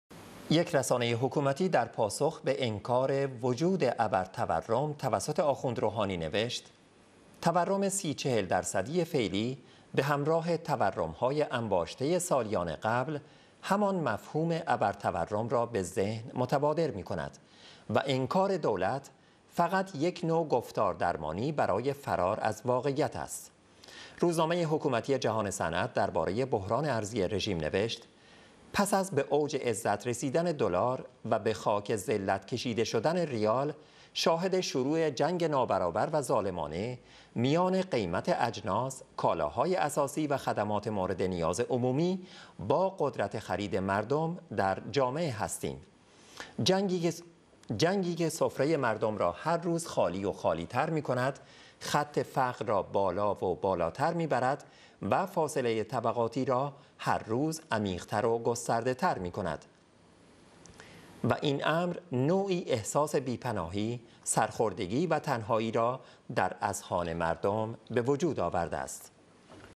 افزایش تورم ابر تورم را تداعی میکند و انکار دولت گفتار درمانی است، یک رسانه حکومت
یک رسانه‌حکومتی در پاسخ به انکار وجود ابرتورم توسط آخوند روحانی نوشت: تورم ۳۰، ۴۰ درصدی فعلی به همراه تورم‌های انباشته‌ی سالیان قبل همان مفهوم «ابرتورم» را به ذهن متبادر می‌کند و انکار دولت، فقط یک نوع «گفتاردرمانی» برای فرار از واقعیت است.
روزنامه حکومتی جهان صنعت در باره بحران ارزی رژیم نوشت: پس از به اوج عزت رسیدن دلار و به خاک ذلت کشیده شدن ریال، شاهد شروع جنگ نابرابر و ظالمانه میان قیمت اجناس، کالاهای اساسی و خدمات مورد نیاز عمومی با قدرت خرید مردم در جامعه هستیم؛ جنگی که سفره مردم را هر روز خالی و خالی‌تر می‌کند، خط فقر را بالا و بالاتر می‌برد و فاصله طبقاتی را هرروز عمیق‌تر و گسترده‌تر می‌کند و این امر نوعی احساس بی‌پناهی، سرخوردگی و تنهایی را در اذهان مردم به وجود آورده است.